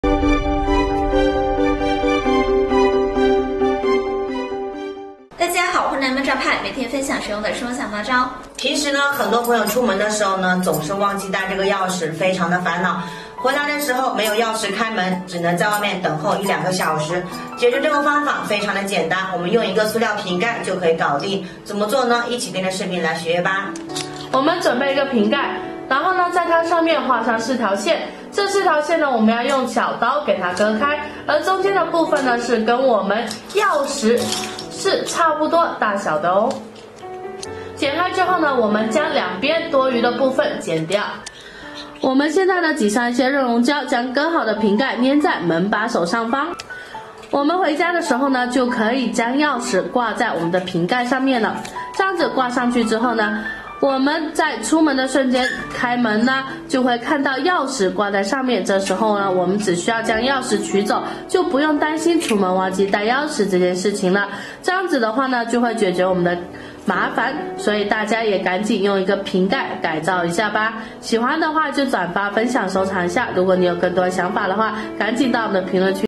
出门忘记带钥匙不用怕，只需一个塑料瓶盖，立马解决难题，太棒了
出门忘记带钥匙不用怕，只需一个塑料瓶盖，立马解决难题，太棒了,本视频由妙招派原创提供,25507次播放,好看视频是由百度团队打造的集内涵和颜值于一身的专业短视频聚合平台